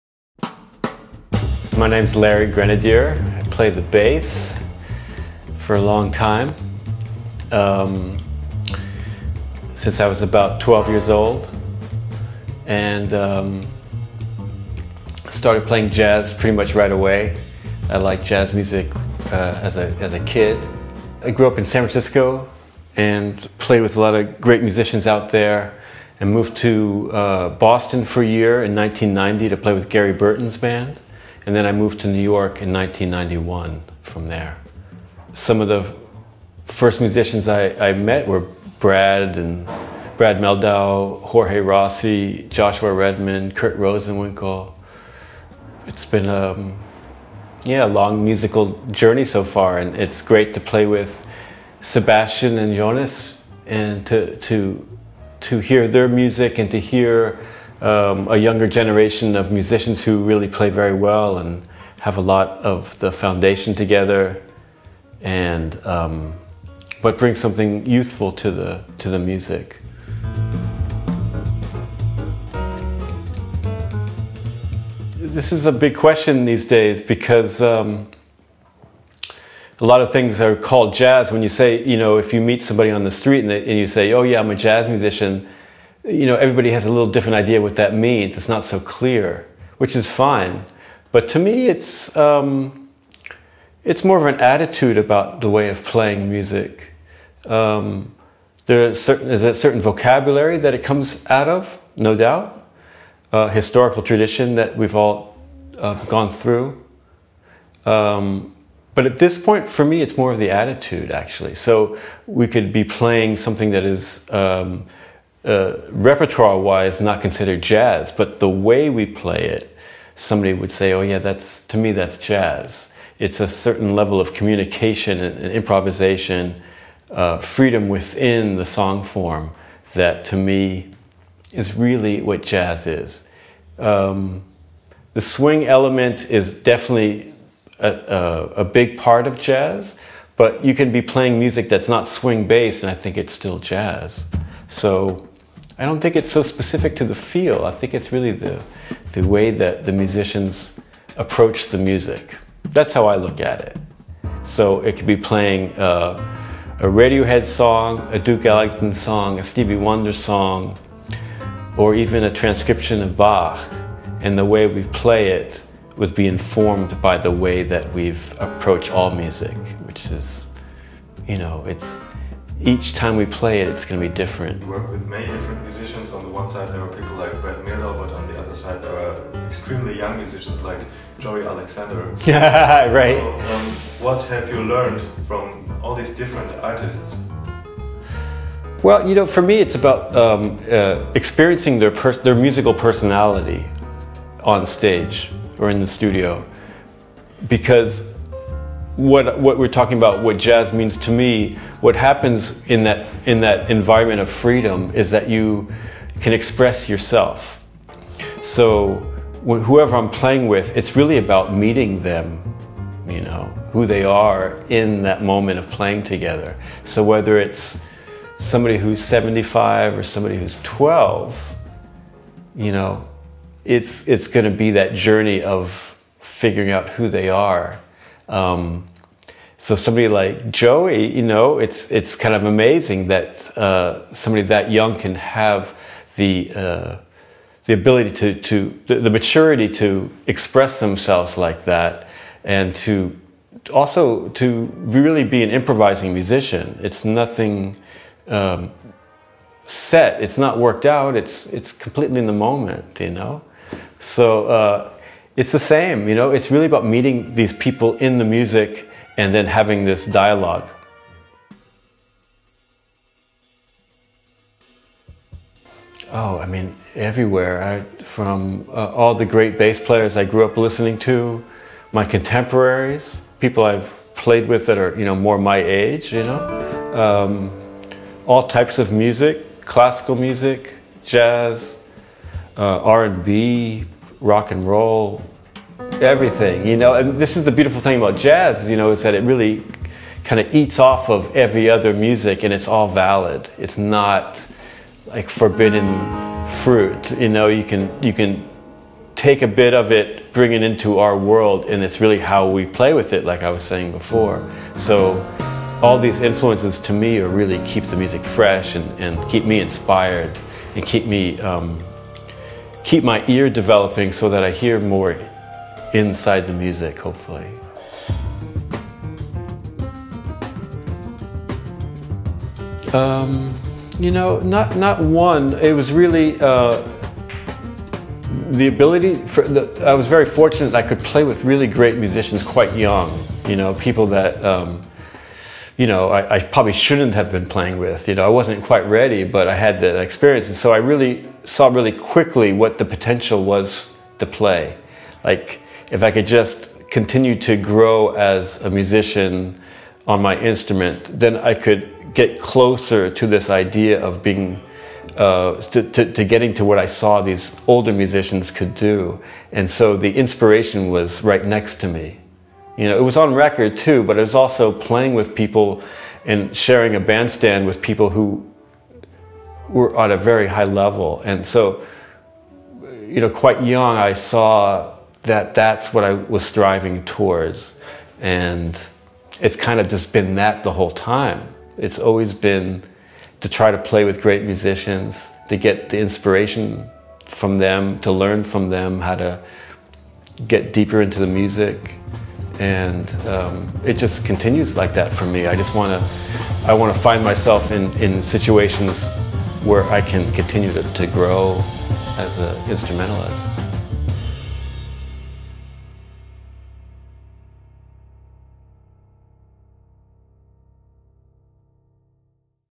Introducing: Larry Grenadier
The famous american bass player, Larry Grenadier, played with artists like Brad Mehldau, Pat Metheny, Joshua Redman or Stan Getz. He talks about his career, his vision on jazz and more!

Video by Julian Camargo.